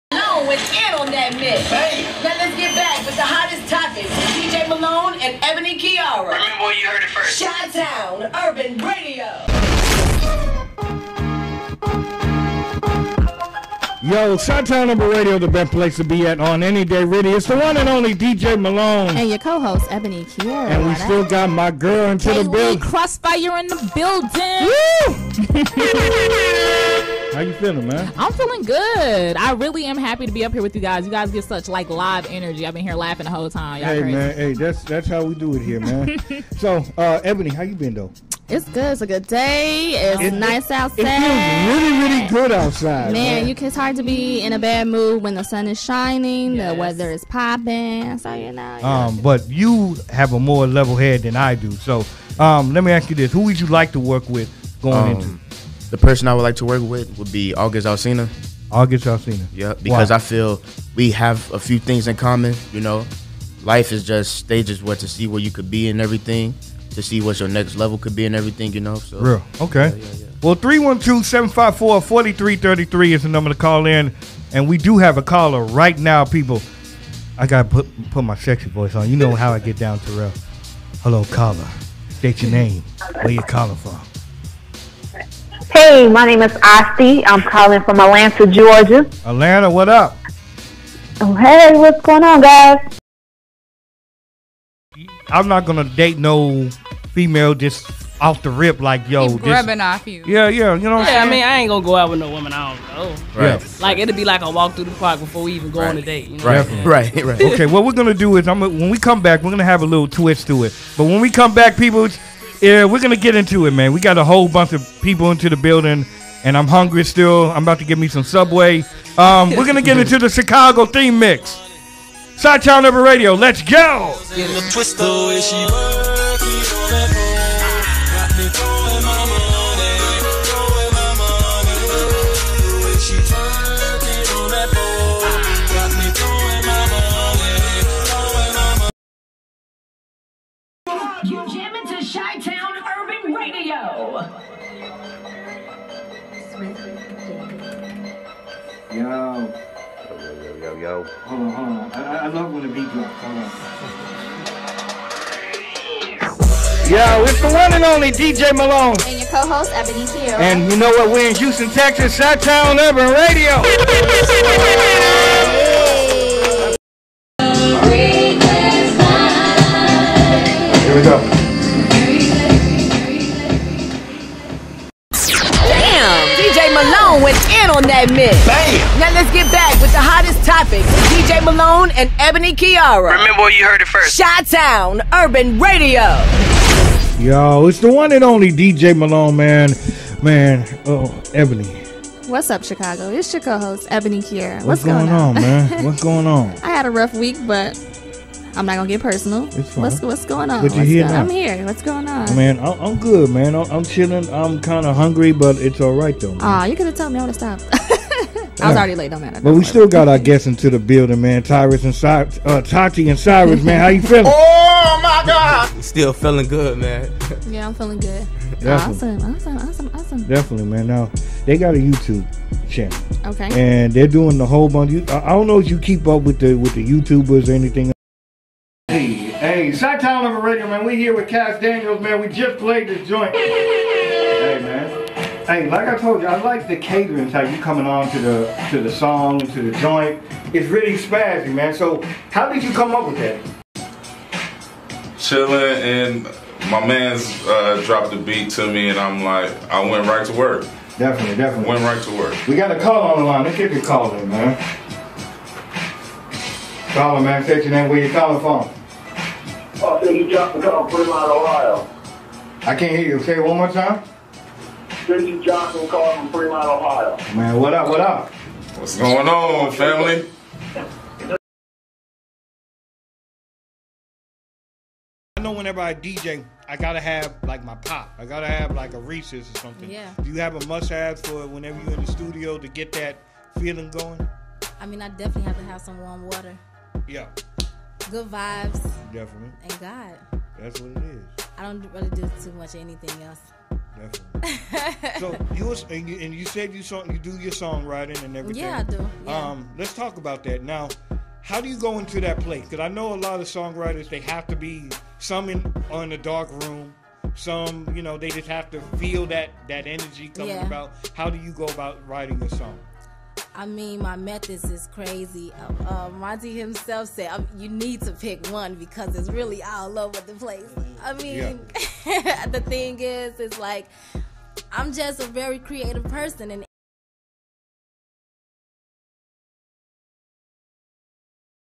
DJ MALONE - AirCheck - Chi Town Urban Radio
Hear the sounds From DJ Malone on Chi-Town Urban Radio as he interviews guests
CHITOWNURBANRADIO.COM
@djmalonepro ig